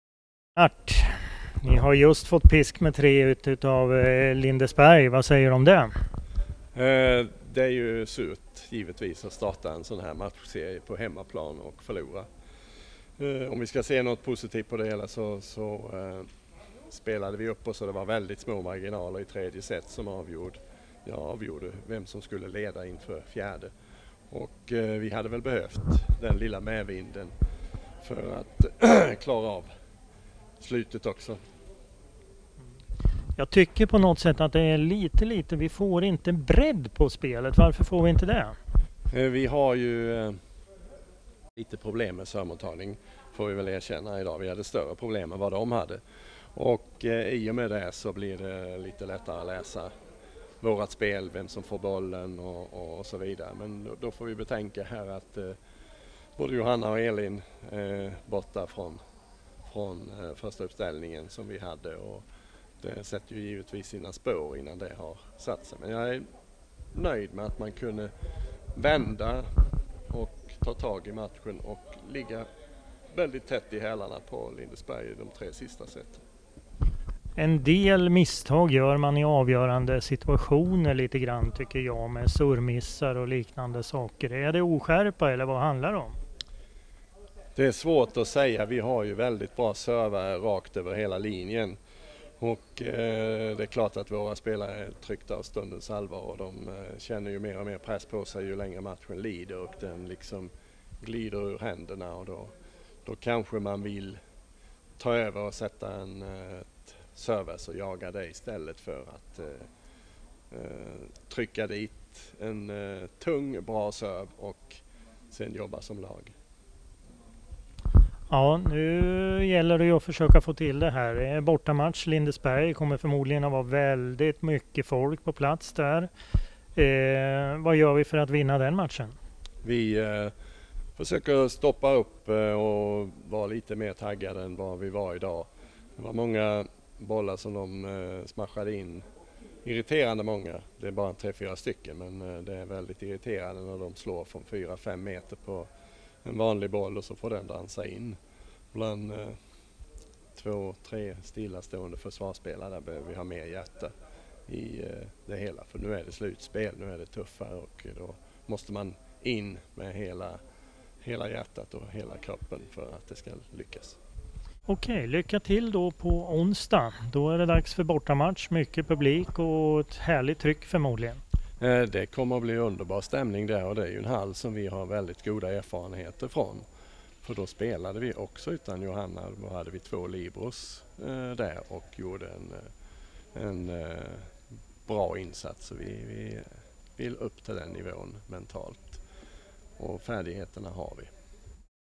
Lennart Larsson efter första semifinalen
Intervju med KVKs tränare Lennart Larsson efter den första semifinalen mot Lindesberg.